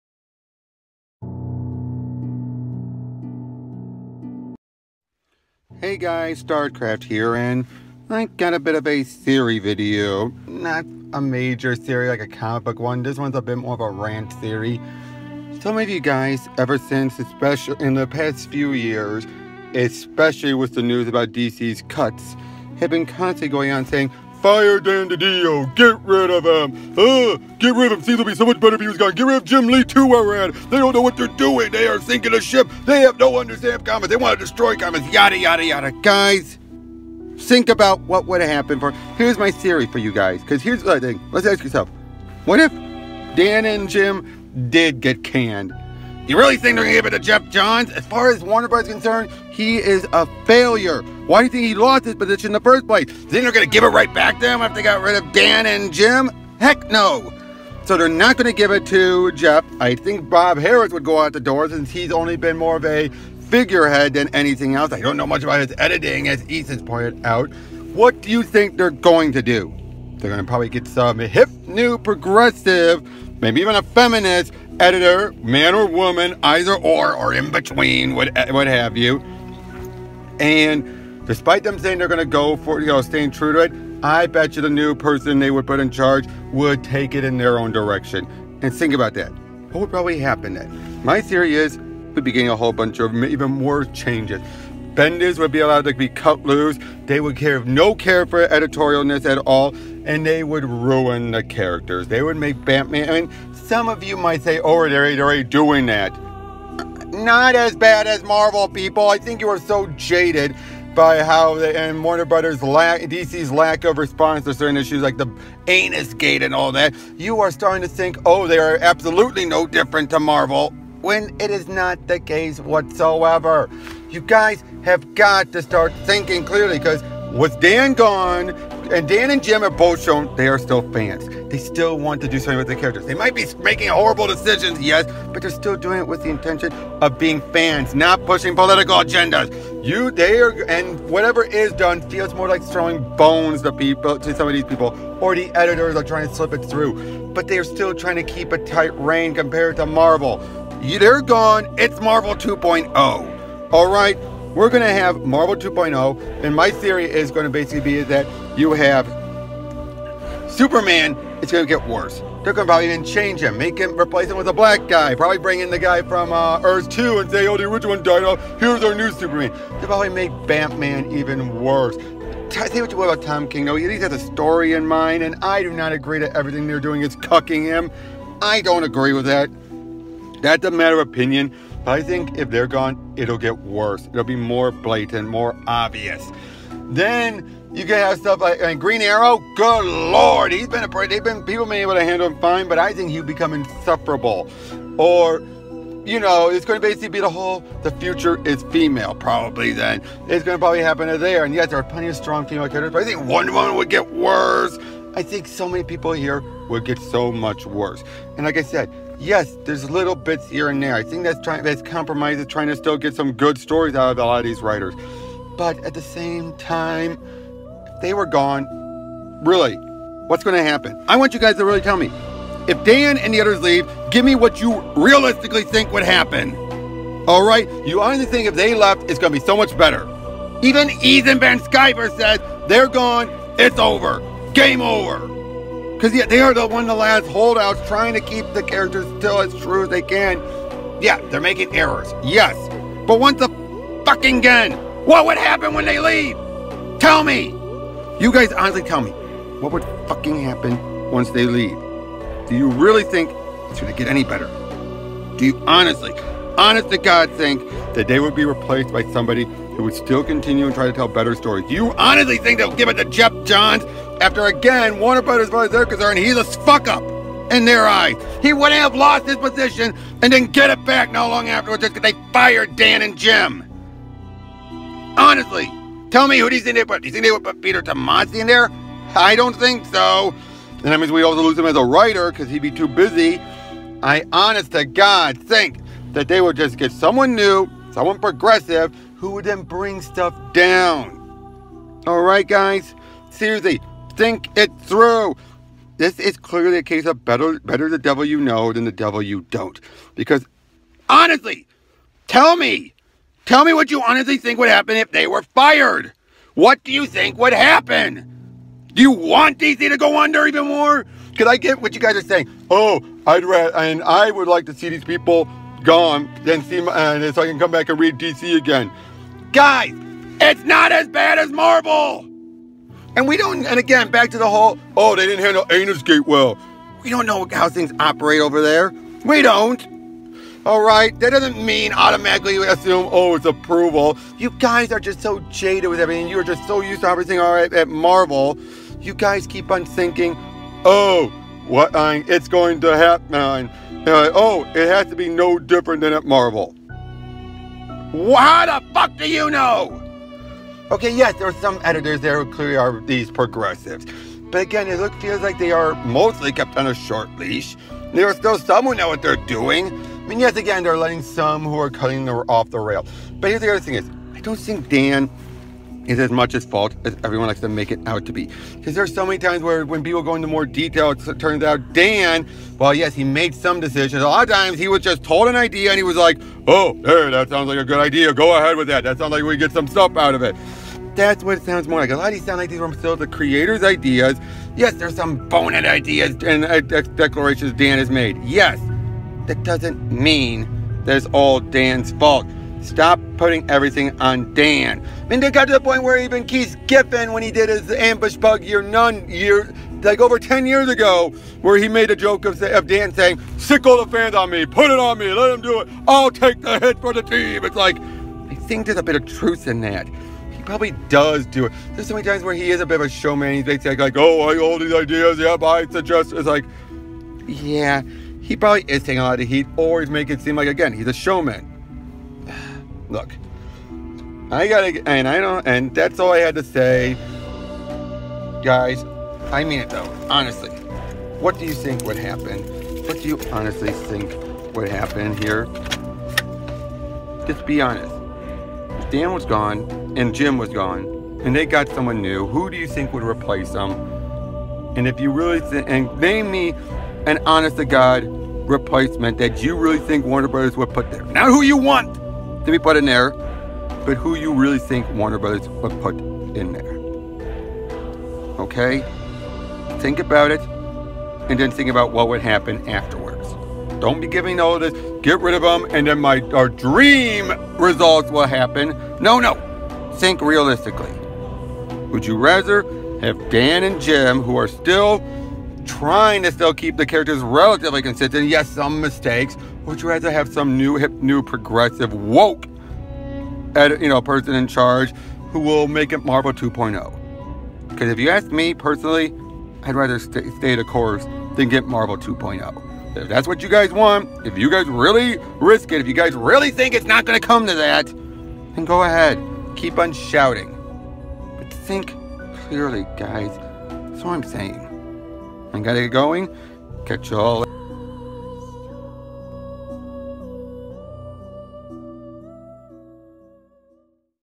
Rant/Theory From the Multiverse: If Didio and Lee Were Fired...What Next?
Okay Guys this is really getting out of hand! I know people don't like Dan Didio (and to a lesser extent Jim Lee) but in all honesty! What would happen, REALLY happen, if they left! Not what we WANT to see happen but what would most likely happen! It won't be pretty if you really think about it!